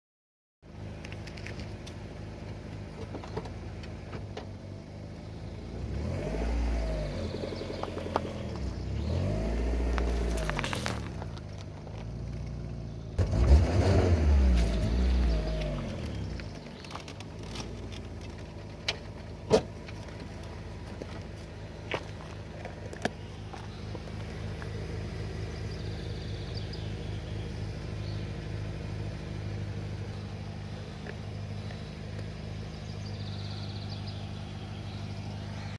How to make an orange juice with car?
Hi, I will show you an experiment. Do you know how to make an orange juice? You need just orange and car. Have a knife day :)